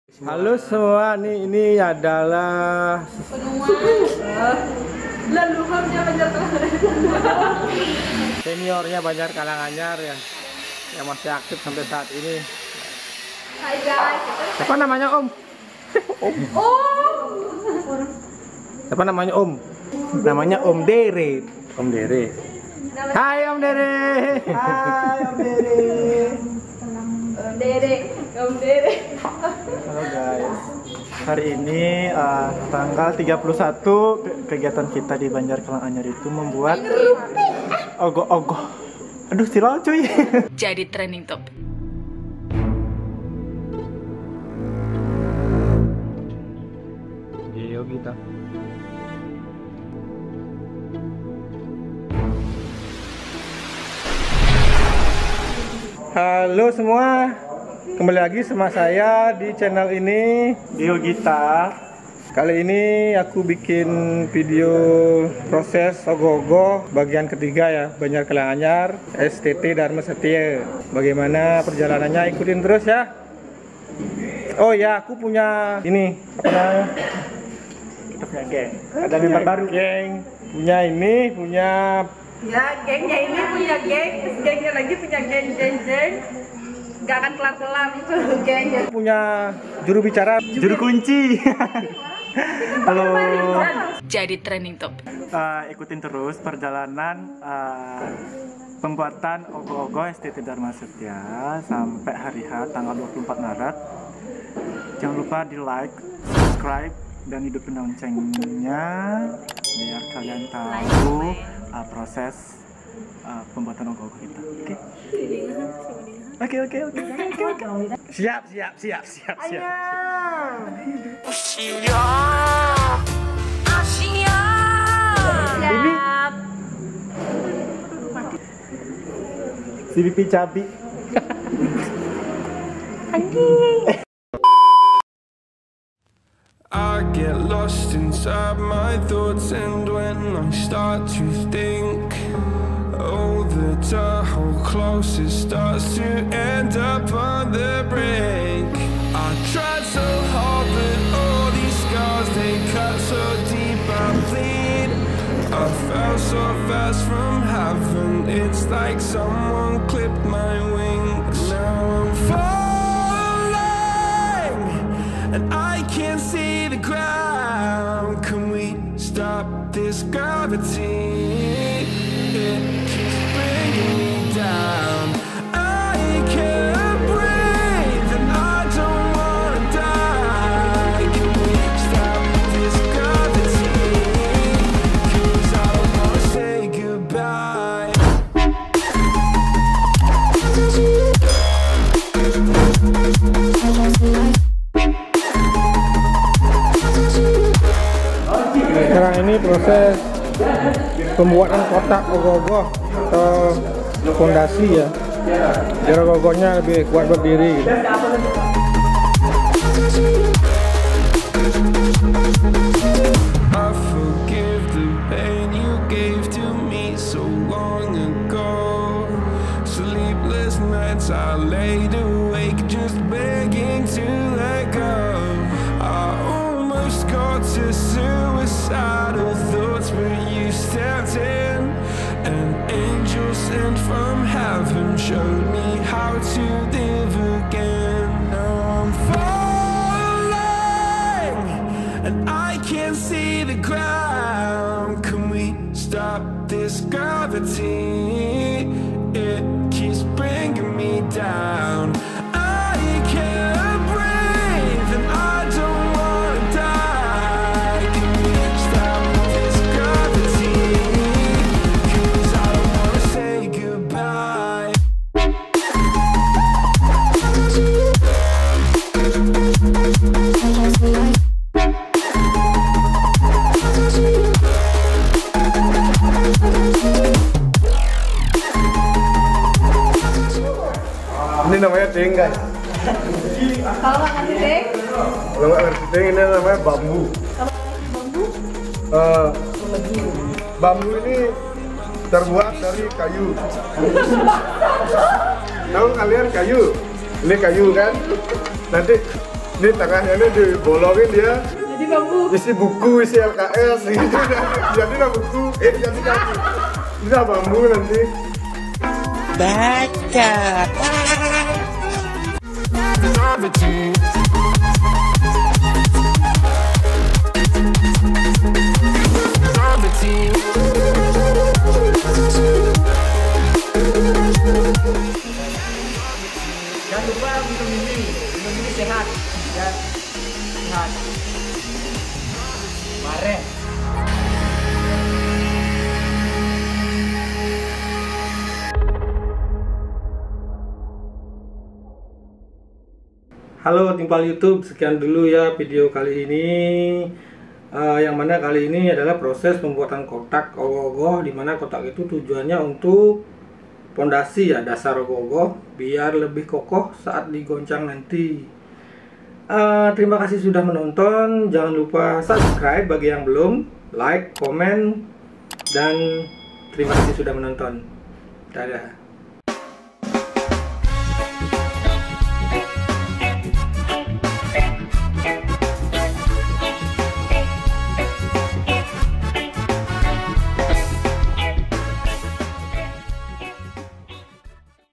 Bikin Ogoh - ogoh Proses Part 3 Br Kalanganyar Jimbaran
hai terimakasi sudah berkunjung perkenalkan nama saya sugita dari bali

saya mencoba menyalurkan hobi bernyanyi dan bermain musik disini ngevlog juga

kali ini saya membuat video proses pembuatan ogoh-ogoh part 3 banjar kalanganyar jimbaran bali 2020